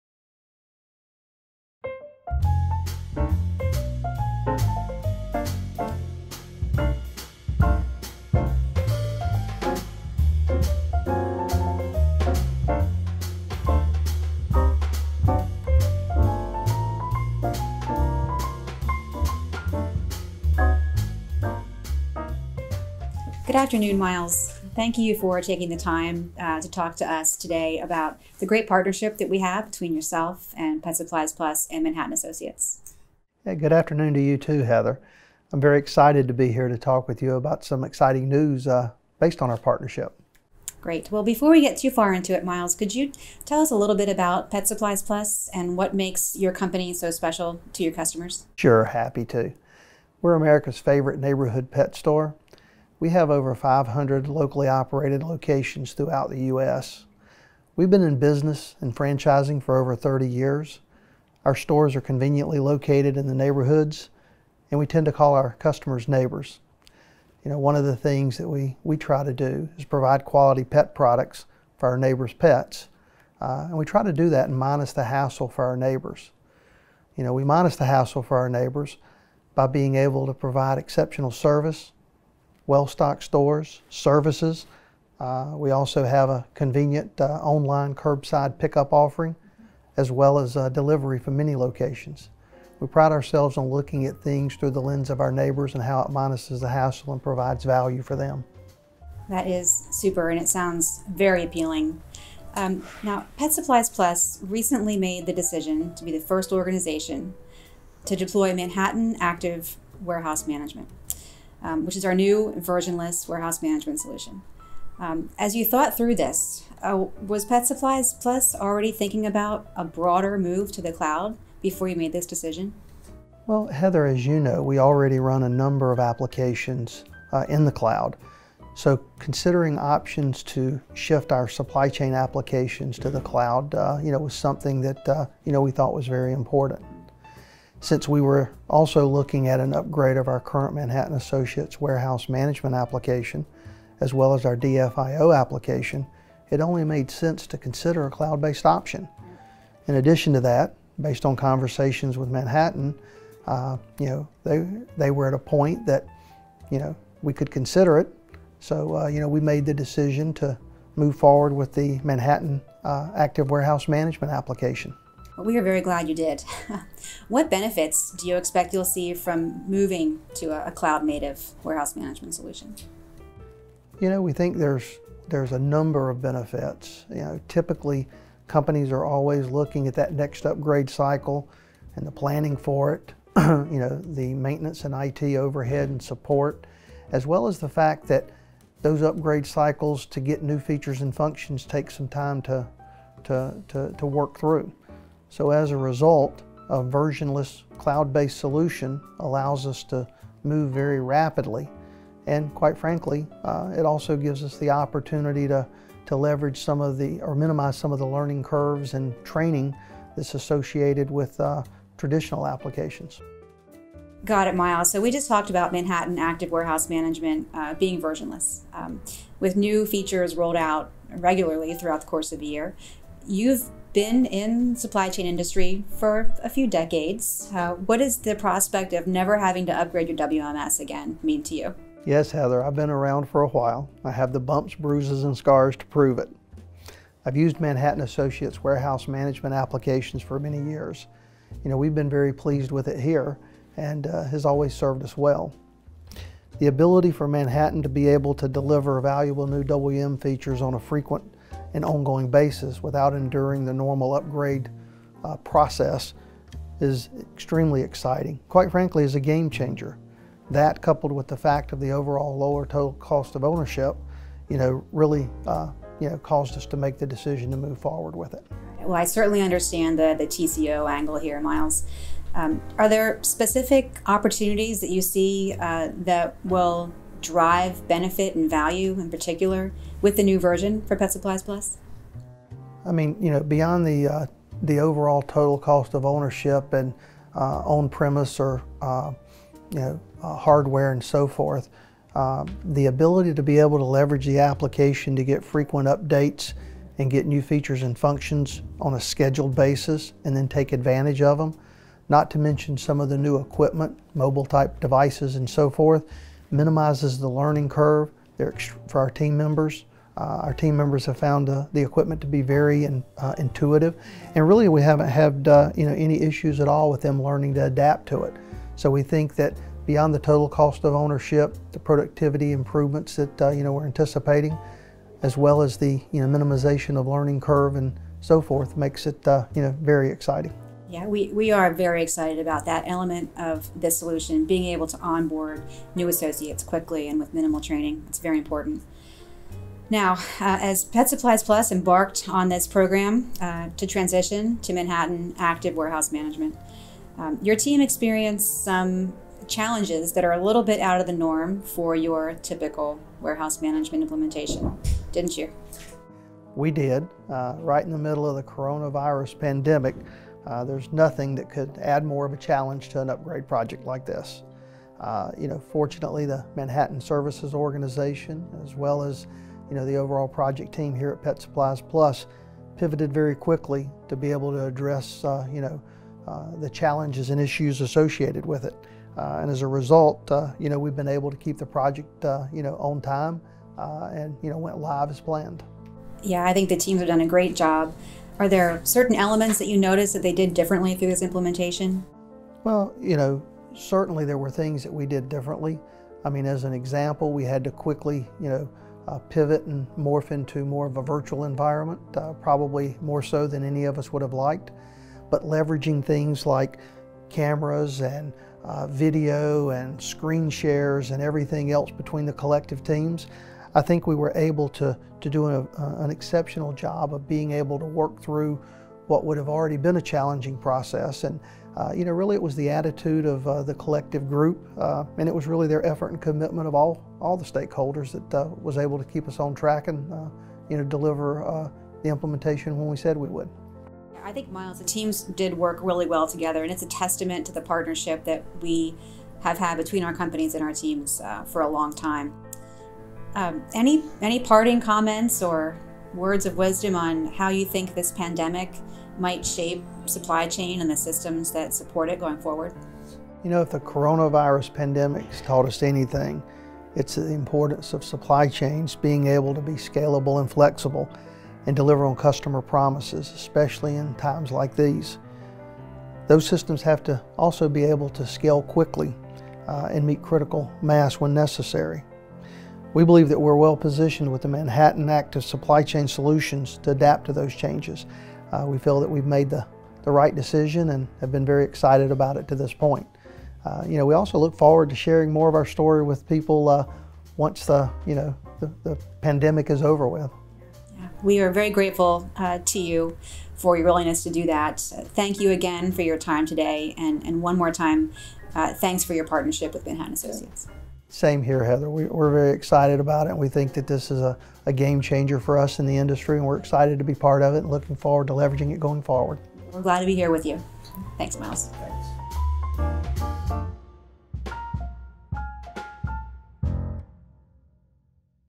Pet Supplies Plus Chooses Manhattan Active® Warehouse Management
Why did Pet Supplies Plus trust our cloud-native Manhattan Active Warehouse Management? Because it’s the only WMS that can deliver the agility, scalability and groundbreaking innovation it needs as a platform for success. Watch our video to learn more.